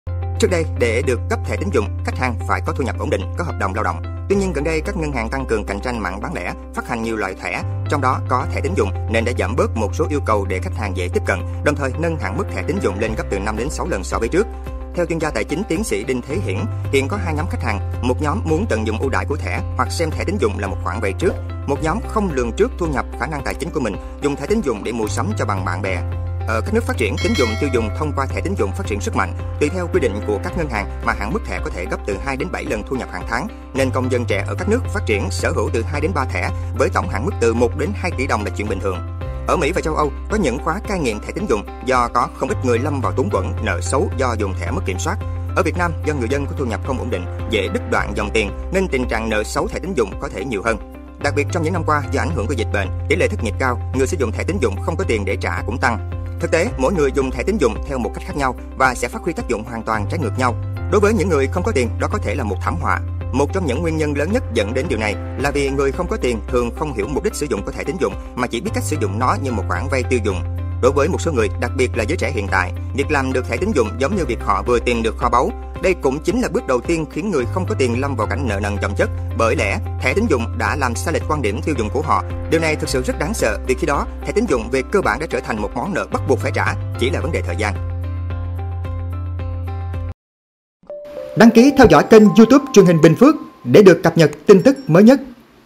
Người trẻ dễ vỡ nợ vì thẻ tín dụng |BPTV
Việc dễ dàng mở thẻ tín dụng, sử dụng thẻ nhưng lại không có kế hoạch quản lý chi tiêu khiến nhiều bạn trẻ nợ tiền, mất khả năng chi trả...

Các chương trình nổi bật:
Cập nhật thêm nhiều thông tin tại:
💎 Fanpage: